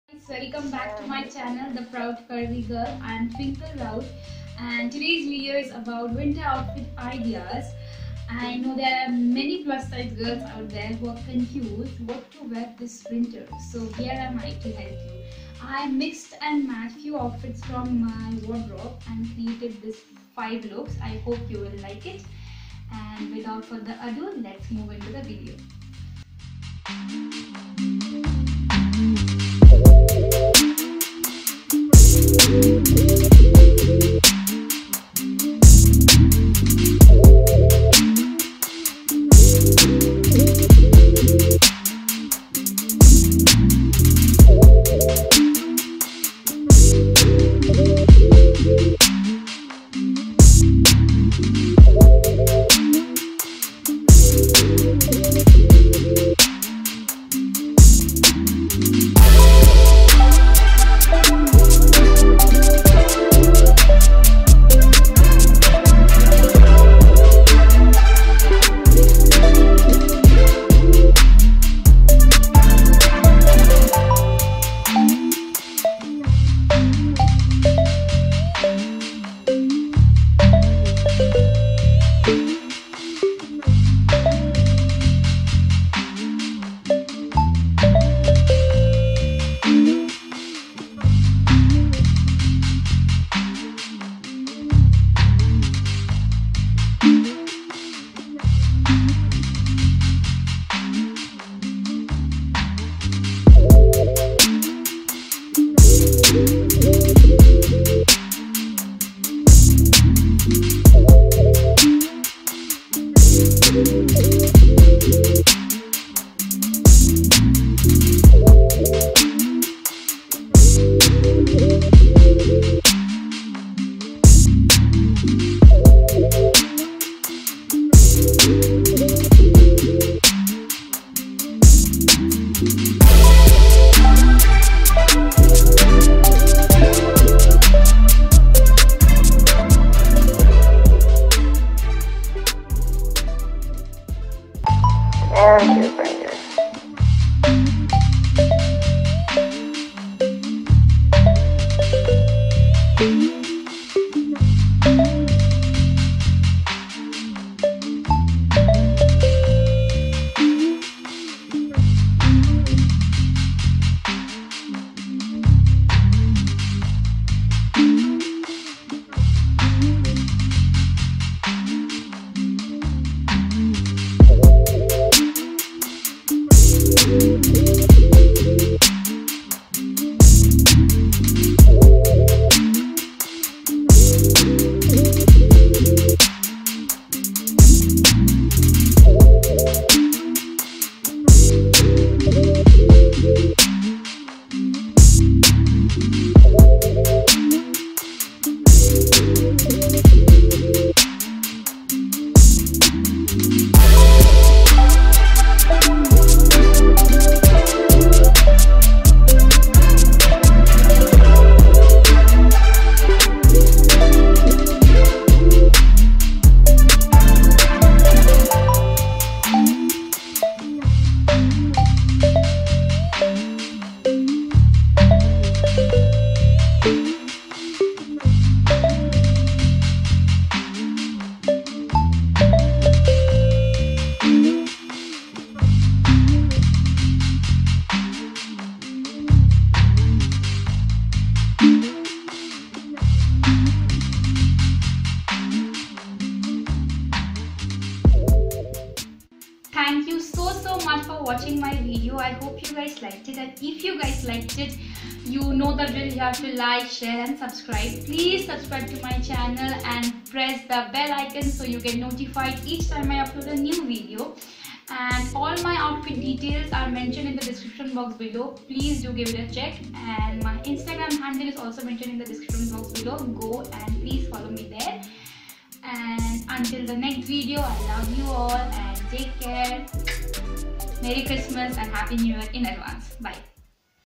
PLUS SIZE WINTER OUTFIT IDEAS| PLUS SIZE CHRISTMAS OUTFITS
Hey guys!
i hope this help you guys while getting decked up during this season . i hope you like it! MERRY CHRISTMAS &HAPPY NEW YEAR IN ADVANCE .
Twinkle.
outfit details-
outfit 1--
shoe-metro plaza ,kolkata
outfit2-
jeans-pantaloons fashion
scarf-local market in darjeeling
outfit 3:-
skirt-romwe fashion
sweater-H&M
earrings- mom :p
outfit 4-
jeans-westsidestore
chain -forever21
 photography &videography -Ruchira Naskar Ayesha Guha,Debanita Guha